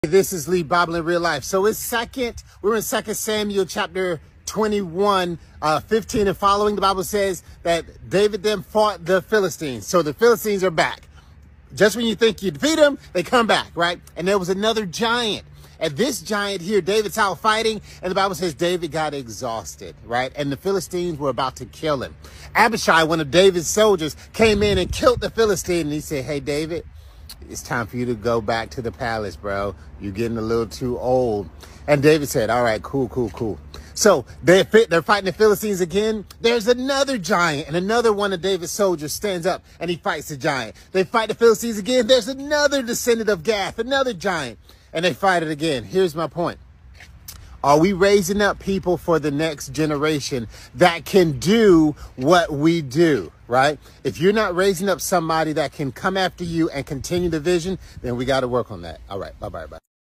Are we raising up the next generation of leaders...
Are we raising up the next generation of leaders, you will get older is there someone that can come behind you continue the fight 2 Sam 21